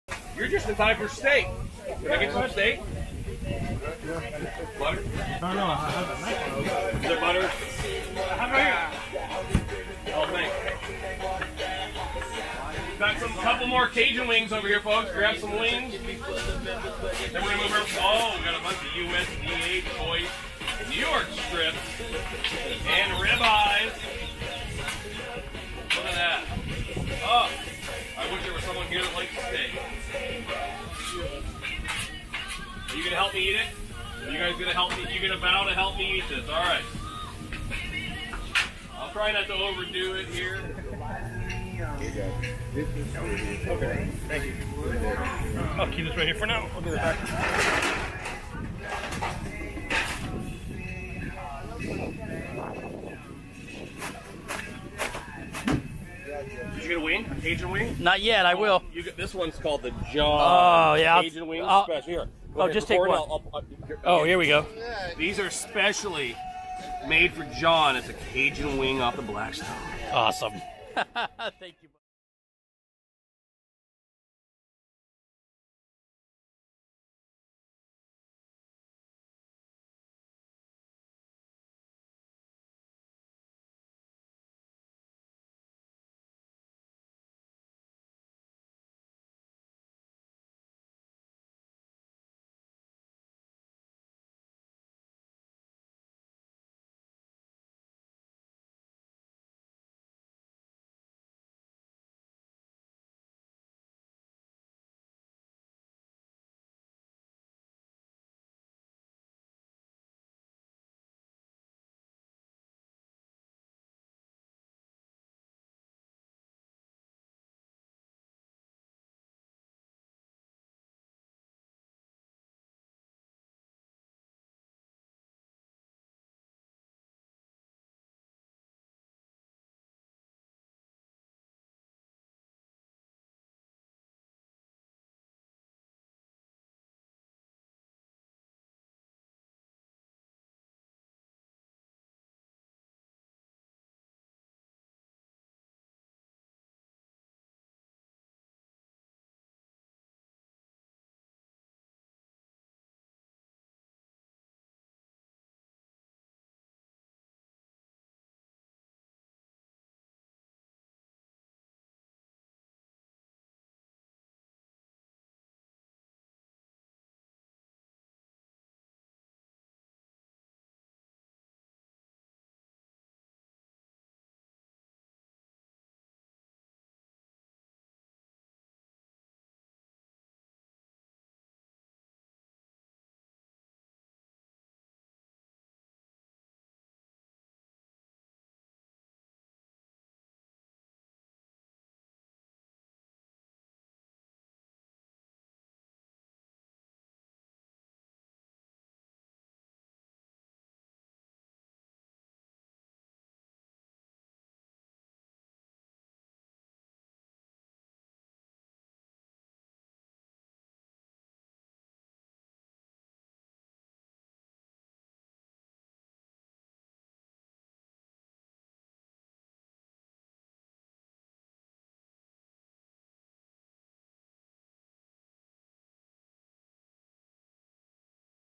Blackstone Griddle Tour - Todd Toven Okeechobee Fl 5-17-19
I got to meet Griddle Master Todd Toven at the Blackstone Griddle More Tour. He was the nicest guy he let me film a little video with my new DJI OSMO Pocket.